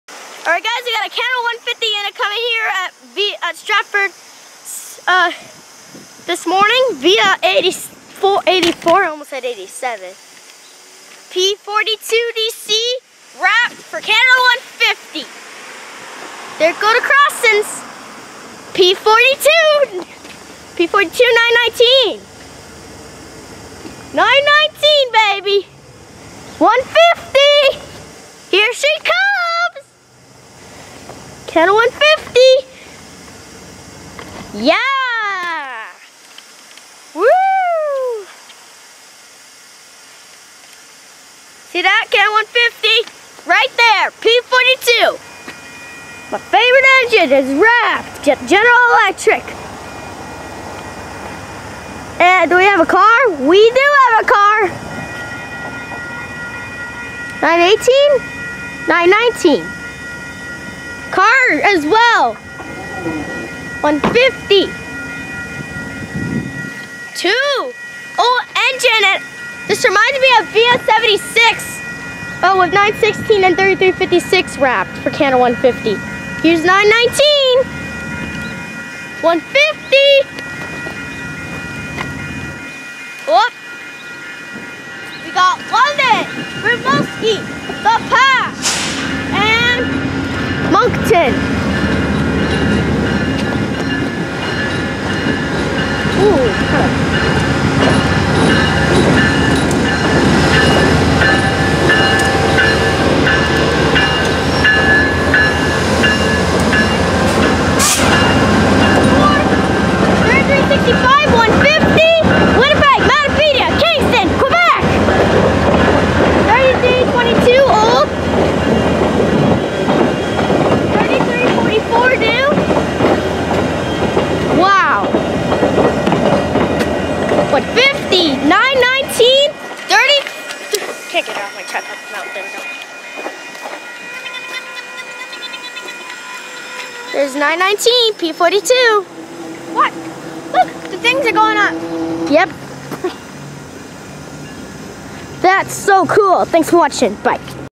CANADA 150 UNIT AND CAR!!!!!!!!!!!!!!!!!! VIA 84 Stratford April 1, 2017 [HD]
GE P42DC 919 (wrapped) LRC 3365 (wrapped)